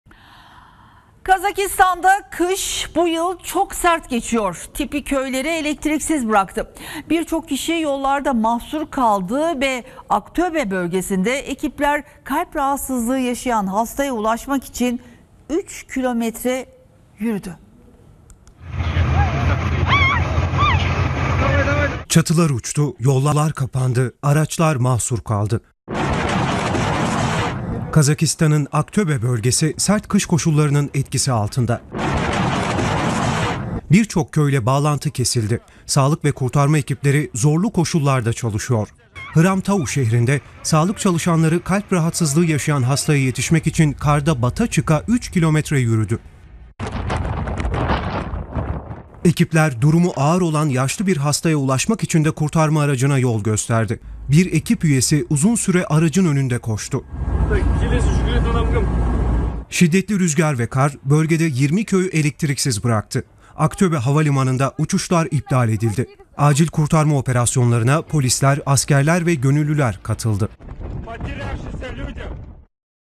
Kazakistan'da Kış Çetin Geçiyor-TRT HABER-10.02.2024
Evlerin çatıları uçuruyor, ağaçlar devriliyor, elektrik kabloları kırılıyor.

Kazakistan’ın Aktöbe bölgesinde şiddetli rüzgar üç gündür etkisini gösteriyor.

Kuvvetli rüzgar,  bölgede 20'den fazla köyü elektriksiz bıraktı.

Kötü hava koşulları nedeniyle Aktobe Havalimanı'nda uçuşlar ertelendi.